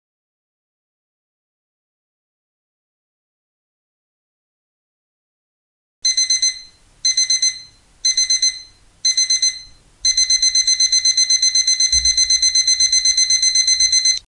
6 Second Timer Countdown No Music With Alarm
6 second timer countdown no music with alarm ⏰. Set a timer for 6 second without music with alarm. Countdown timer without music for classroom, school, youth group, workout, etc.